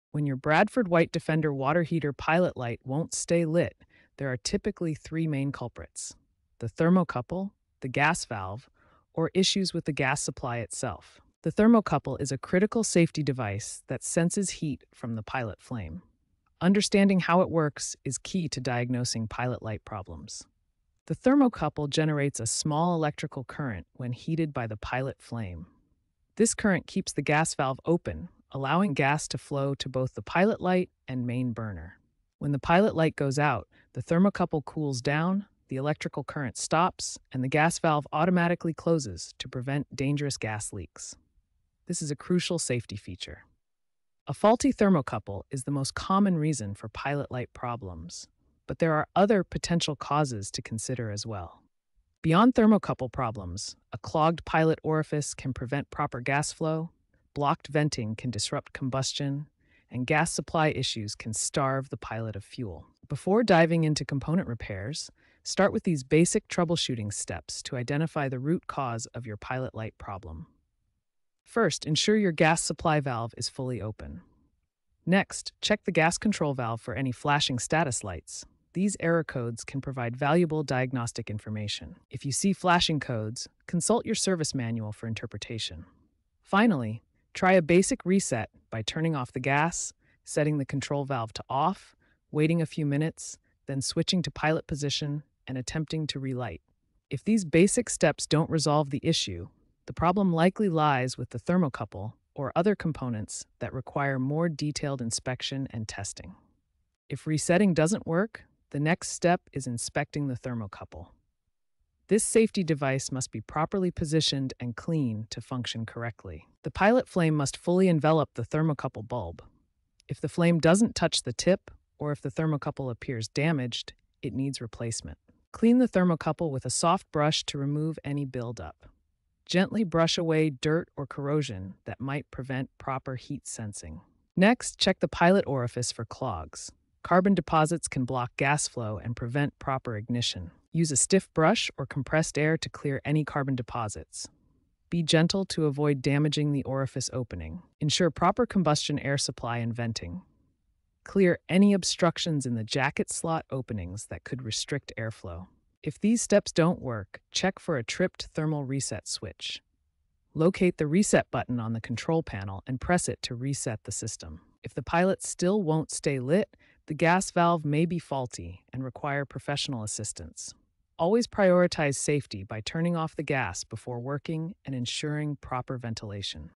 Fix Bradford White Defender Water Heater Pilot Light Won’t Stay Lit – Thermocouple & Valve Solutions
Discover effective solutions to fix your Bradford White Defender water heater's pilot light issues. Learn about thermocouple and valve troubleshooting tips.